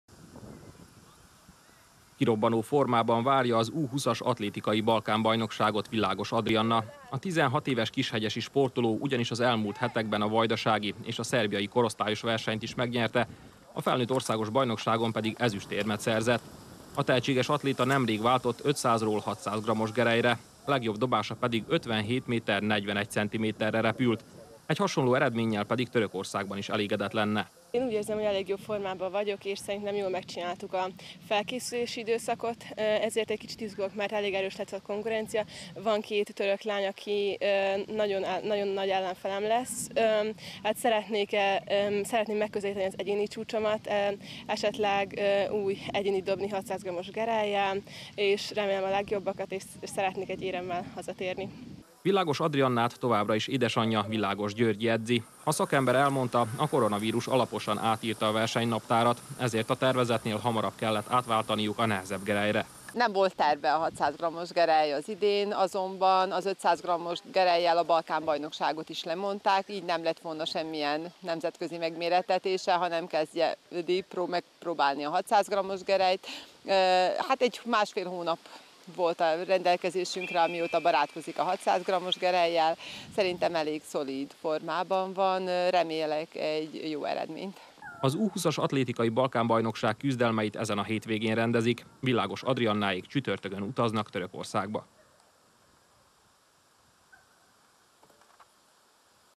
2020.09.07. Éremért utazik a Balkán-bajnokságra Világos Adriana
A gerelyhajításban korosztályos világcsúcstartó lány célja, hogy újabb egyéni rekordot dobjon Törökországban.